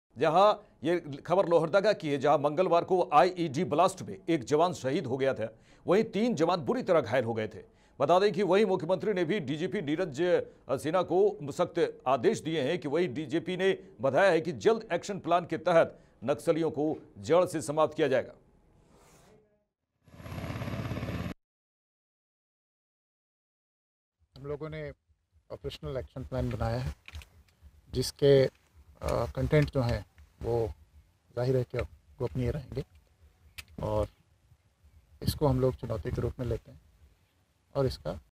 Lohardaga : नक्सलियों के विरोध एक्शन प्लान तैयार, डीजीपी नीरज सिन्हा ने ये कहा, देखें वीडियो
लोहरदगा : नक्सलियों के विरोध एक्शन प्लान तैयार, डीजीपी नीरज सिन्हा ने ये कहा, देखें वीडियो